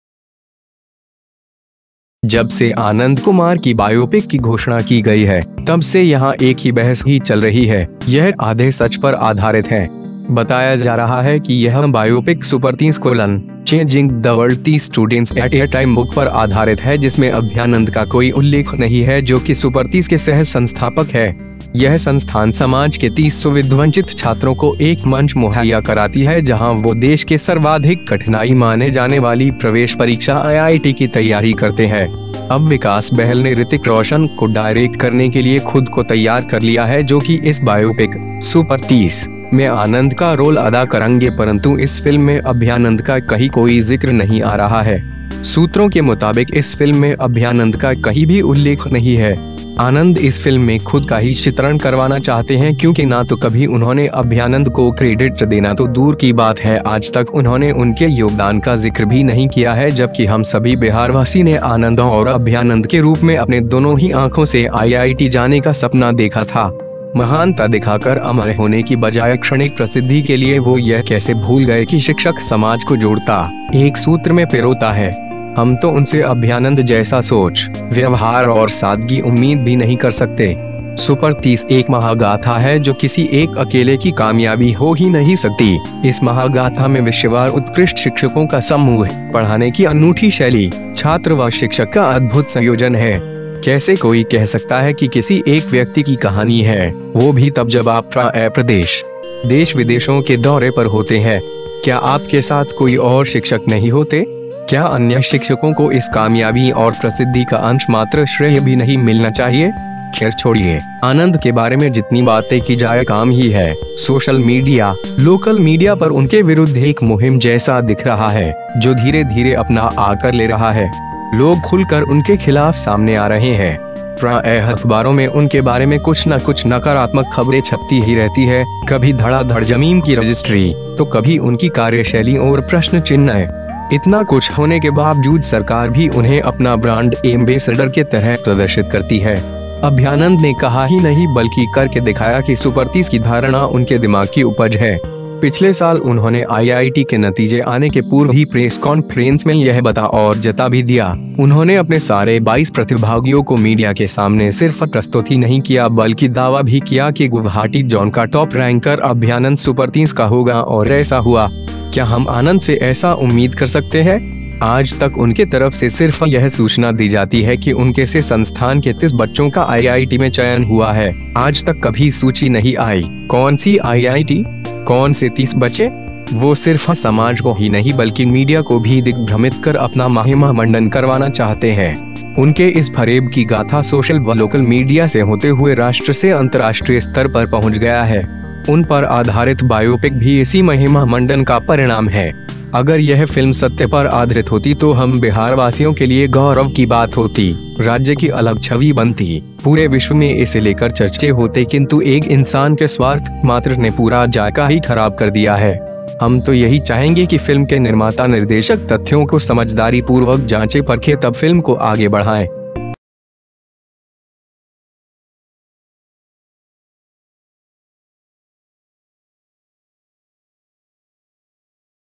रितिक रॉशन व टीम को सच्चाई खुद खोजनी चाहिए', बोले 'super 30' के सह संस्थापक
जबसे आनंद कुमार की बायोपिक की घोषणा की गई है, तब से यहां एक ही बहस ही चल रही है, यह आधे सच पर आधारित है। बताया जा रहा है कि यह बायोपिक 'सुपर 30: चेंजिंग द वर्ल्ड 30 स्टूडेंट्स ऐट ऐ टाइम' बुक पर आधारित है जिसमें अभयानंद का कोई उल्लेख नही है जो कि 'सुपर 30' के सह-संस्थापक है। यह संस्थान समाज के 30 सुविधवंचित छात्रों को एक मंच मुहैया कराती है जहाँ वो देश के सर्वाधिक कठिनाई माने जाने वाली प्रवेश परीक्षा आईआईटी की तैयारी करते है।

अब विकास बहल ने रितिक रॉशन को डायरेक्ट करने के लिए खुद को तैयार कर लिया है जो कि इस बायोपिक (सुपर 30) में आनंद का रोल अदा करंगे परंतु इस फ़िल्म में अभयानंद का कही कोई जिक्र नही आ रहा है। सूत्रों के मुताबिक 'इस फ़िल्म में अभयानंद का कही भी उल्लेख नही है। आनंद इस फ़िल्म में खुद का ही चित्रण करवाना चाहते हैं क्योंकि ना तो कभी उन्होंने अभयानंद को क्रेडिट देना तो दूर की बात है आज तक उन्होंने उनके योगदान का जिक्र भी नही किया है जबकि हम सभी बिहारवासी ने 'आनंद और अभयानंद' के रूप में अपने दोनों ही आंखों से आईआईटी जाने का सपना देखा था। महानता दिखा कर अमर होने की बजाय क्षणिक प्रसिद्धि के लिए वो यह कैसे भूल गए कि शिक्षक समाज को जोड़ता,एक सूत्र में पिरोता है। हम तो उनसे अभयानंद जैसा सोच, व्यवहार और सादगी उम्मीद भी नही कर सकते।
'सुपर 30' एक महागाथा है जो किसी एक अकेले की कामयाबी हो ही नही सकती, इस महागाथा में विषयवार उत्कृष्ट शिक्षकों का समूह, पढ़ाने की अनूठी शैली, छात्र व शिक्षक का अद्भुत संयोजन है। कैसे कोई कह सकता है कि किसी एक व्यक्ति की कहानी है, वो भी तब जब आप प्रायः प्रदेश, देश-विदेशों के दौरे पर होते है। क्या आपके साथ कोई और शिक्षक नही होते? क्या अन्य शिक्षकों को इस कामयाबी और प्रसिद्धि का अंश मात्र श्रेय भी नही मिलना चाहिए?
खैर छोड़िए, आनंद के बारे में जितनी बाते की जाय काम ही है, सोशल मीडिया, लोकल मीडिया पर उनके विरुद्ध एक मुहिम जैसा दिख रहा है, जो धीरे-धीरे अपना आकर ले रहा है। लोग खुलकर उनके खिलाफ सामने आ रहे है। प्रायः अखबारों में उनके बारे में कुछ न कुछ नकारात्मक खबरे छपती ही रहती है, कभी धड़ा-धड़ जमीन की रजिस्ट्री, तो कभी उनकी कार्यशैली ओर प्रश्न चिन्ह। इतना कुछ होने के बाबजूद सरकार भी उन्हें अपना ब्रांड-एम्बेसडर के तरह प्रदर्शित करती है।

अभयानंद ने कहा ही नही बल्कि कर के दिखाया कि 'सुपर 30' की धारणा उनके दिमाग की उपज है। पिछले साल उन्होंने आईआईटी के नतीजे आने के पूर्व ही प्रेस कॉन्फ्रेंस में यह बता और जता भी दिया, उन्होंने अपने सारे 22 प्रतिभागियो को मीडिया के सामने सिर्फ प्रस्तुत ही नही किया बल्कि दावा भी किया कि गुवहाटी जोन का टॉप रैंकर 'अभयानंद सुपर 30' का होगा और ऐसा हुआ। क्या हम आनंद से ऐसा उम्मीद कर सकते है? आज तक उनके तरफ से सिर्फ यह सूचना दी जाती है कि उनके से संस्थान के 30 बच्चों का आईआईटी में चयन हुआ है, आज तक कभी सूची नही आयी, कौन सी आईआईटी? कौन से 30 बच्चे? वो सिर्फ समाज को ही नही बल्कि मीडिया को भी दिग्भ्रमित कर अपना महिमामंडन करवाना चाहते है। उनके इस फरेब की गाथा सोशल व लोकल मीडिया से होते हुए राष्ट्र से अंतरराष्ट्रीय स्तर पर पहुँच गया है।

उन पर आधारित बायोपिक भी इसी महिमामंडन का परिणाम है। अगर यह फ़िल्म सत्य पर आधरित होती तो हम बिहारवासियों के लिए गौरव की बात होती, राज्य की अलग छवि बनती, पूरे विश्व मे इसे लेकर चर्चे होते किंतु एक इंसान के स्वार्थ मात्र ने पूरा जायका ही खराब कर दिया है। हम तो यही चाहेंगे कि फ़िल्म के निर्माता/निर्देशक तथ्यों को समझदारी पूर्वक जांचे-परखे तब फ़िल्म को आगे बढ़ाएं।